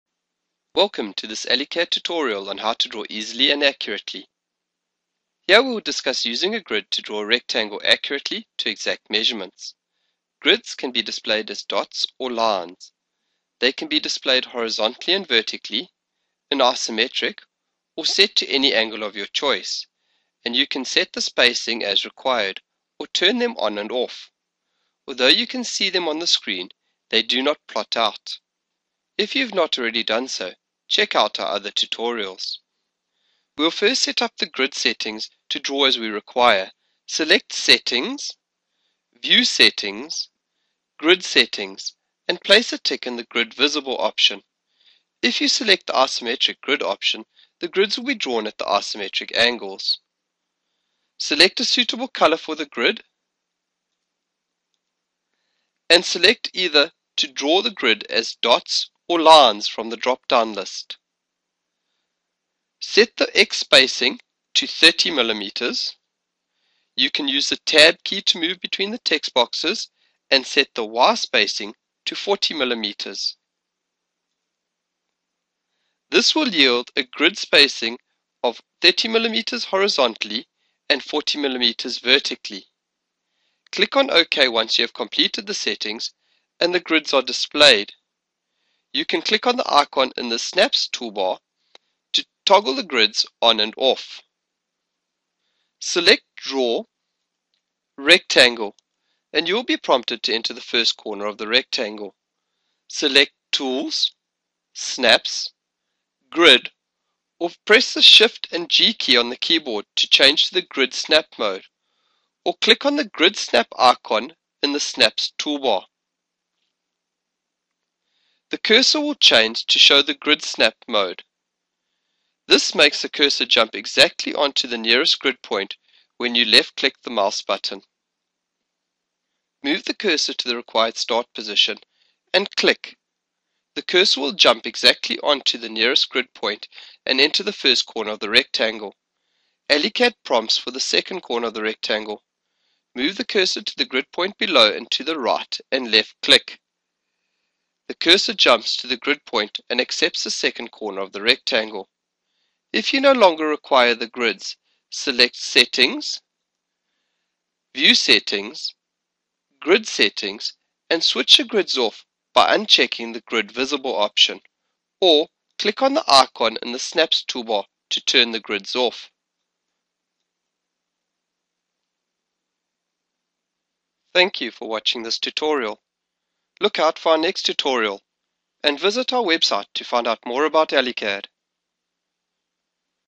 AllyCAD Software: Using Grids
AllyCAD Software: Using Grids to draw a rectangle to exact measurements